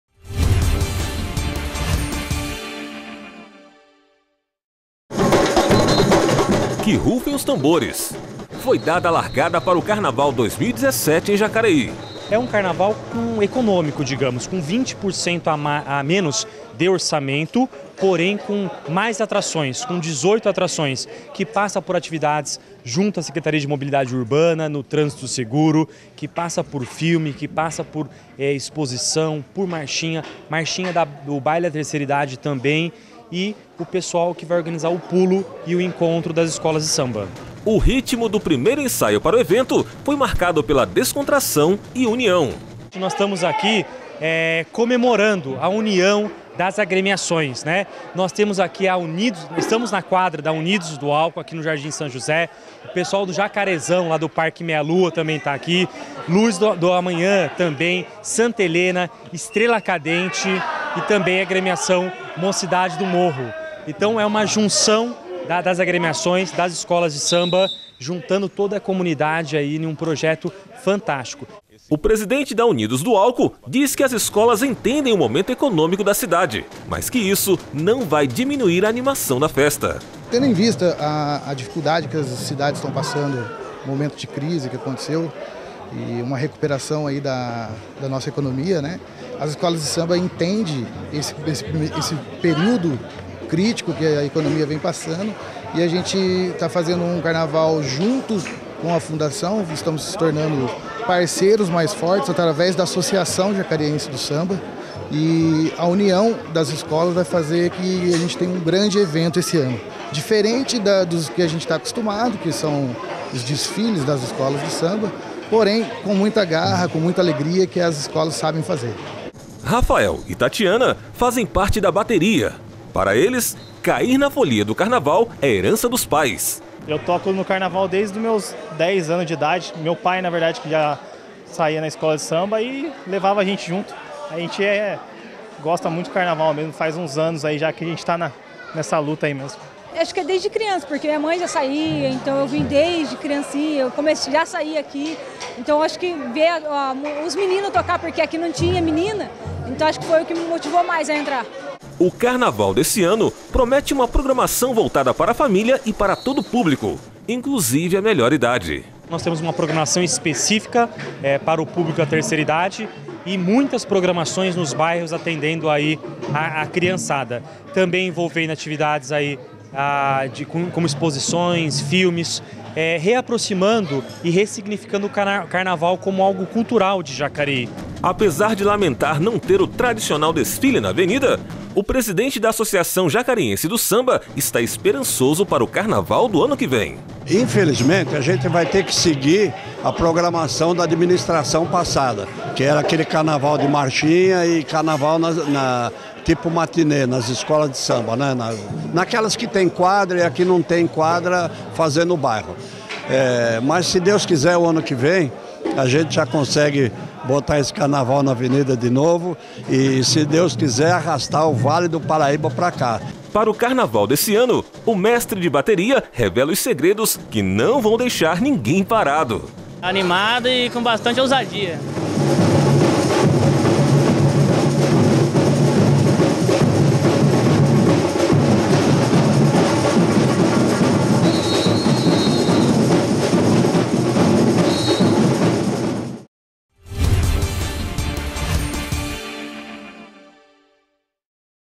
Lançamento 'Carnaval 2017'
Foi dada a largada para o Carnaval 2017 em Jacareí.